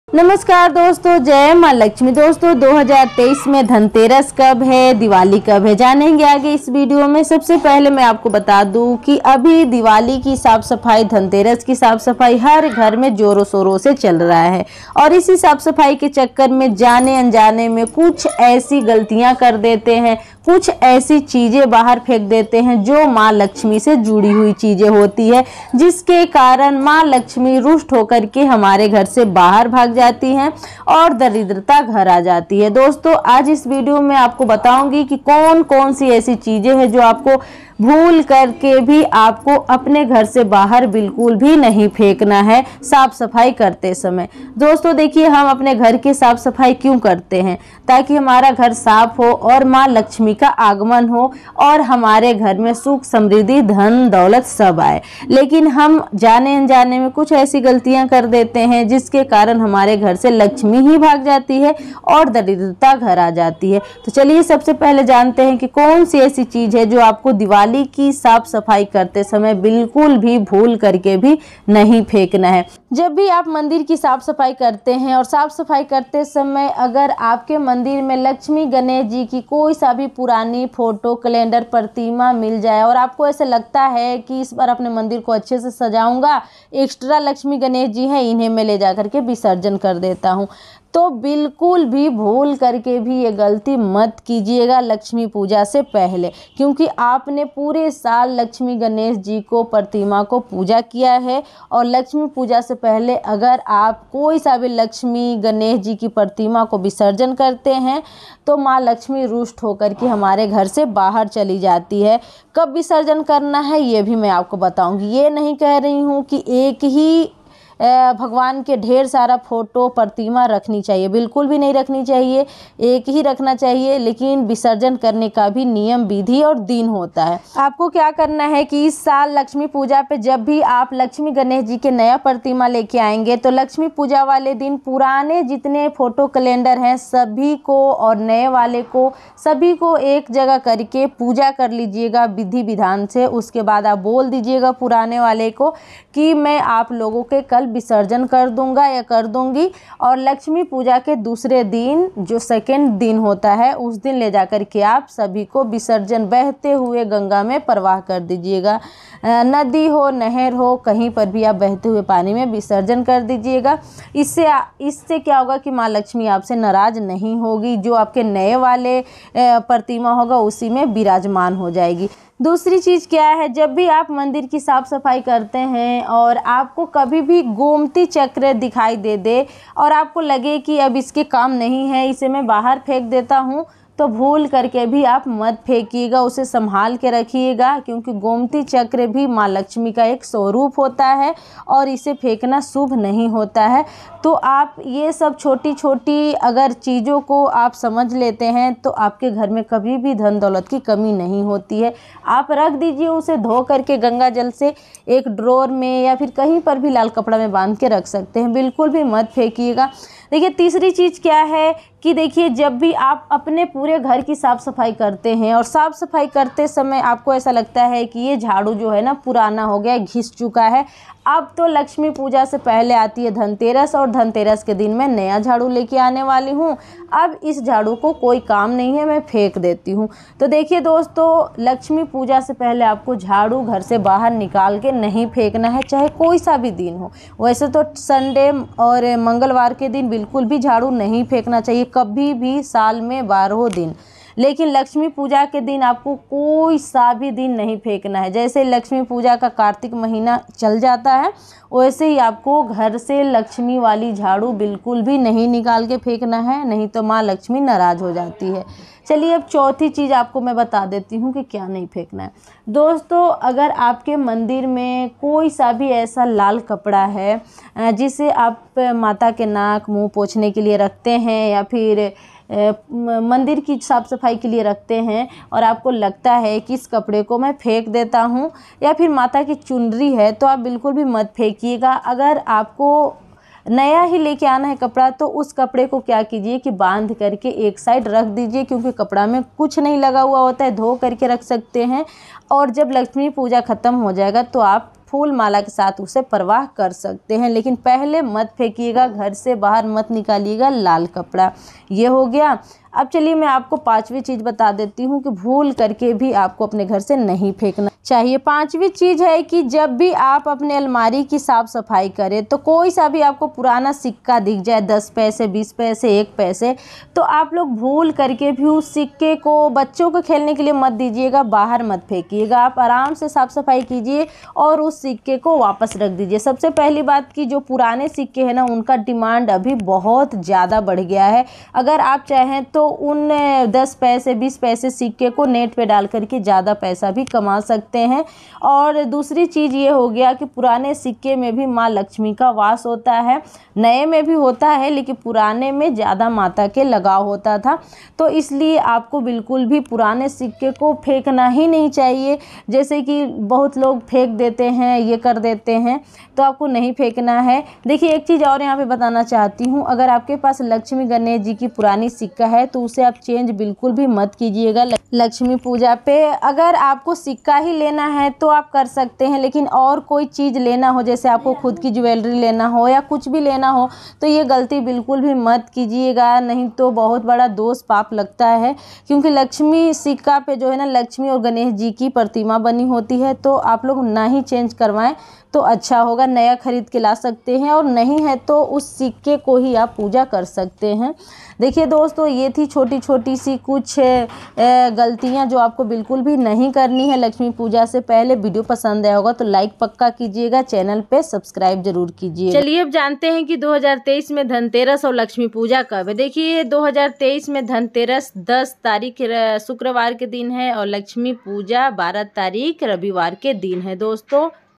Dhanteras kab hai 2023 || दिवाली की सफाई में भूलकर भी मत फेंकना ये 5 चीजें | घर से लक्ष्मी भाग जाएगी
धनतेरस के दिन जरूर खरीदे 1शुभ चीज जिससे आपके घर में पूरे साल माता लक्ष्मी का वास रहें और बरकत होती रहें/ पूजा का शुभ मुहूर्त/खरीदारी का शुभ मुहूर्त /धनतेरस 2023

DIWALI PUJA
Diwali Murti
Lakshmi Ganesh ki Murti
Diwali 2023
 इस वीडियो में आप धनतेरस से जुड़ी हुई संपूर्ण जानकारी इस वीडियो से प्राप्त करेंगेधन्यवाद